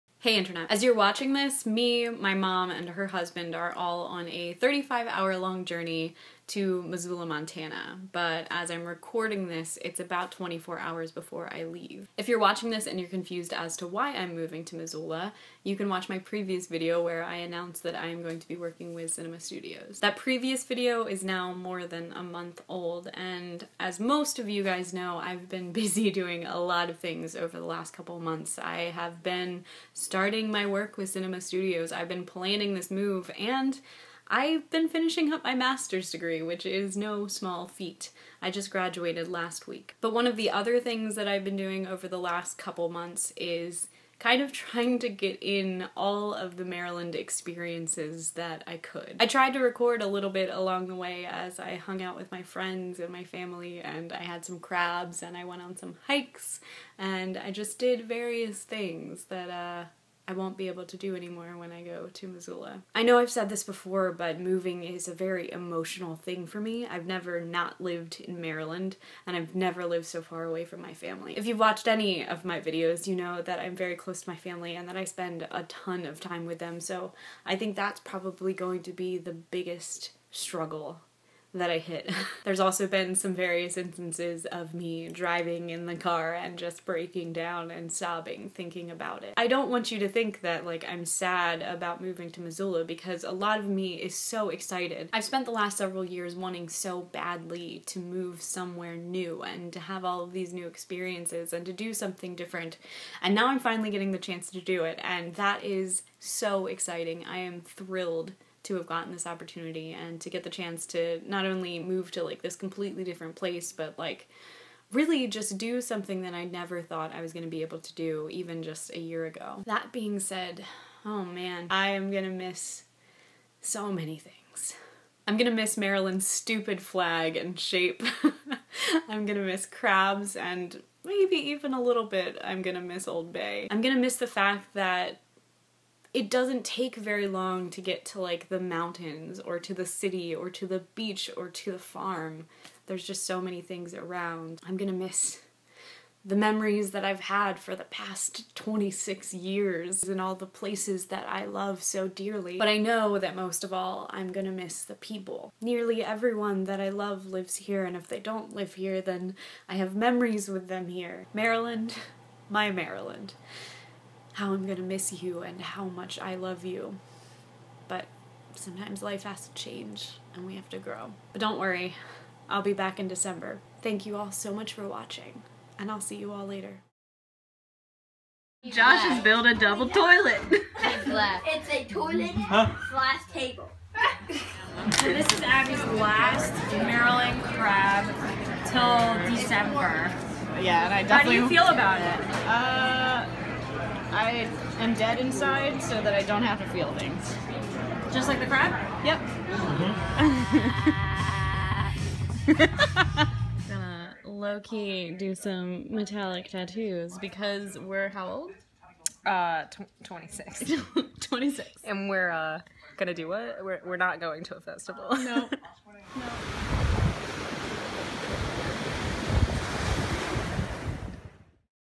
Goodbye, Maryland
I'm in Missoula, Montana now. But I wanted to take a second to look back at what I love and will miss about Maryland.